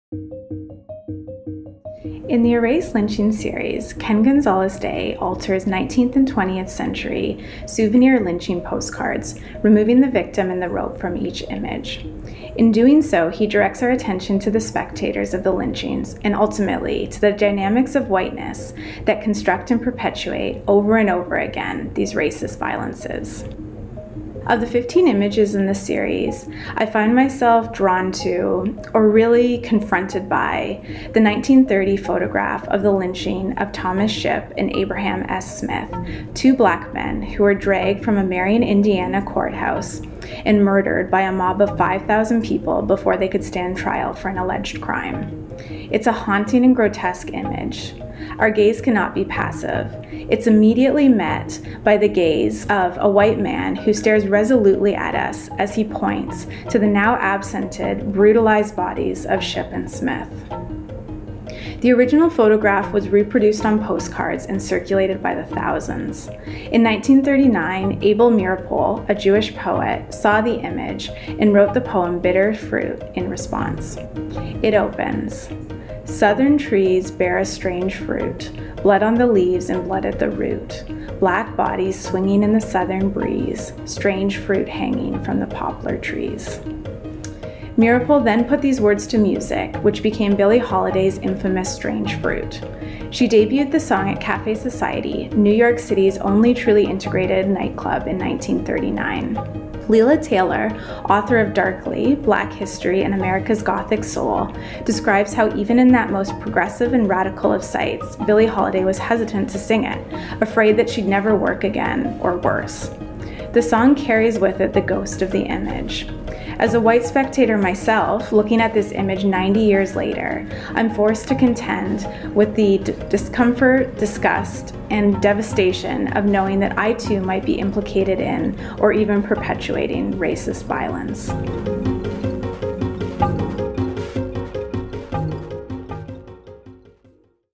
Visual Citizenship | Erased Lynching Series by Ken Gonzales-Day
Assistant Curator Georgia Erger shares background information on Ken Gonzales-Day’s Erased Lynchings. The series of works directly implicate the spectator as participant. The artist gathers nineteenth- and twentieth-century postcards depicting lynchings of Latin Americans, African Americans, Native Americans, Asian Americans, and others, scans them, and then digitally removes the victim and the rope from each image. In doing this he directs our attention to the spectators, who are often pointing or jeering at the (now absented) victim or staring resolutely at the camera. The erasure of the lynched victim paradoxically makes more visible these violent, racist acts, as well as the dynamics of whiteness that systematically erase such historical narratives. The project demands that we examine our own role in such histories. As spectators of these widely and callously circulated images, are we too perpetrating racist violence?